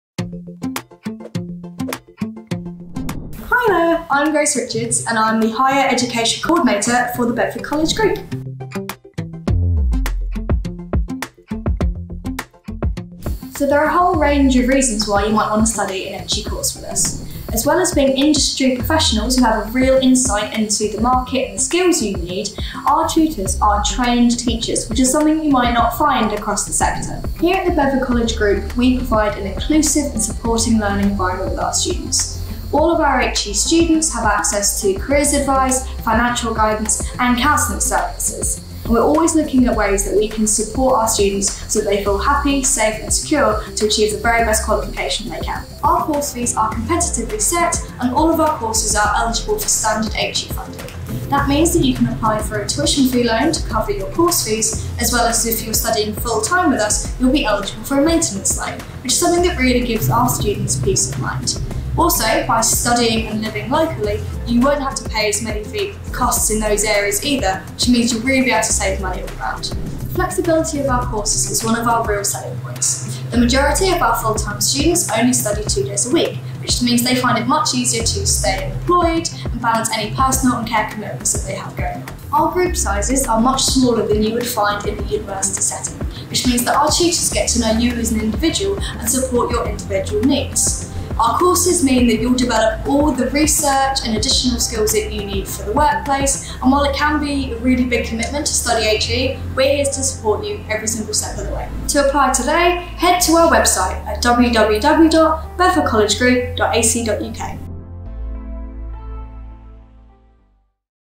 Why Study a Higher Education Course at The Bedford College Group?
In this video, Higher Education Co-ordinator Grace Richards explains why you should study a Higher Education course at The Bedford College Group.